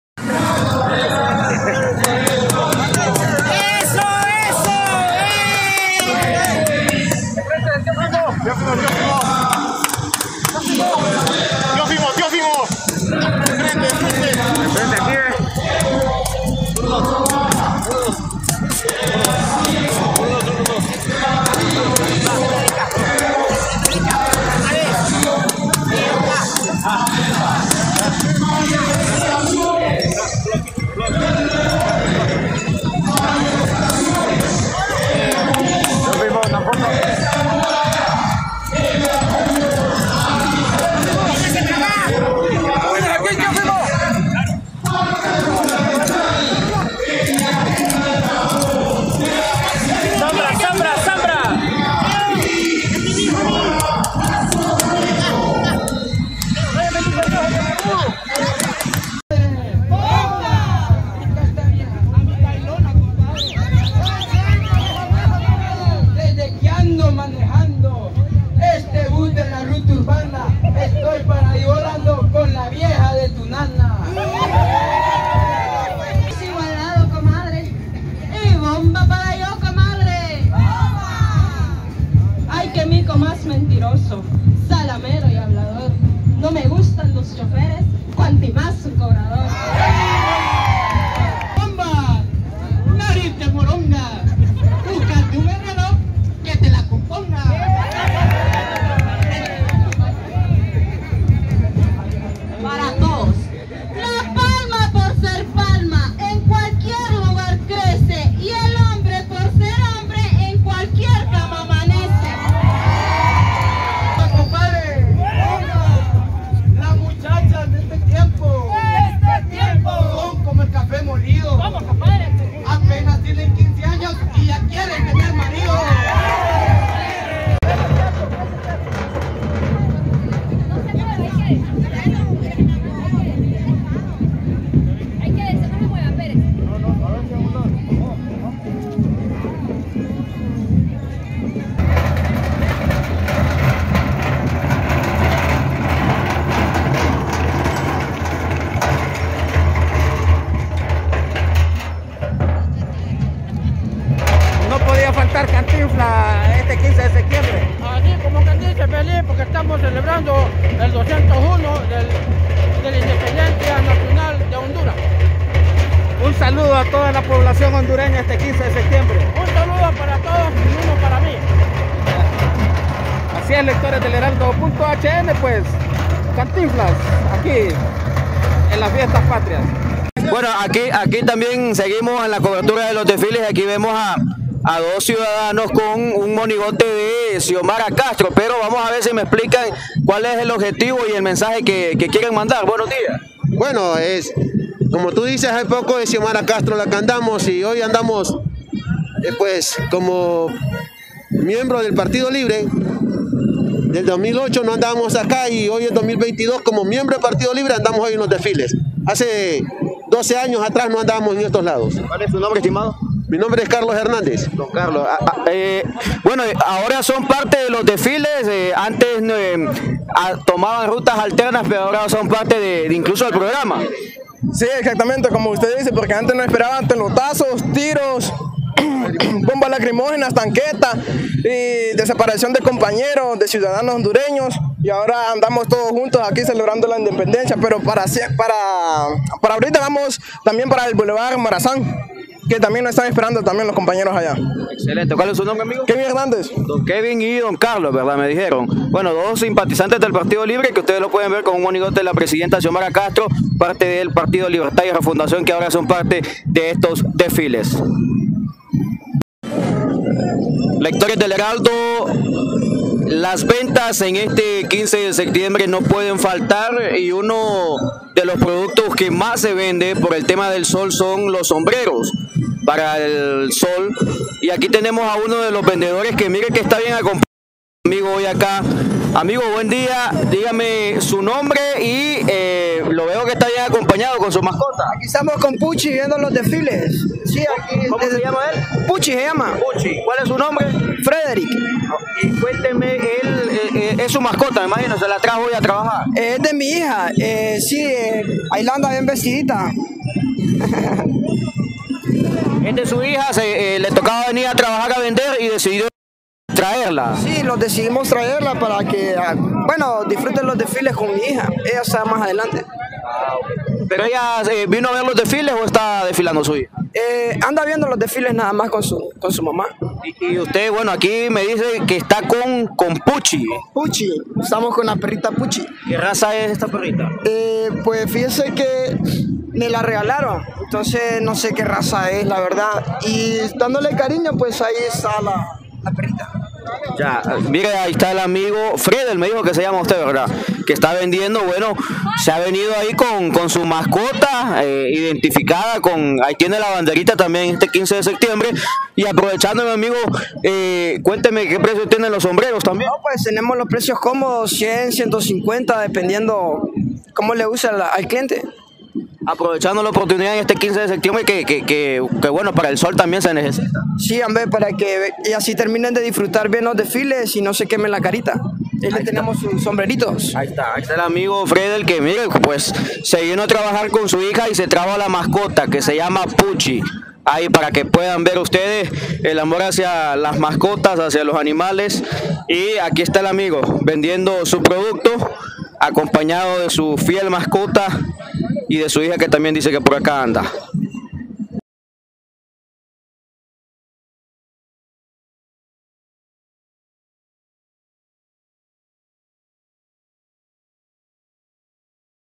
Lo que no viste de los desfiles patrios en Honduras
La increíble voltereta que dio Teófimo en la cancha, el "cantinflas" barrendero y puqui en compañía de su amo vendiendo sombreros... ¡Estas son las imágenes que aún no has visto y que EL HERALDO captó!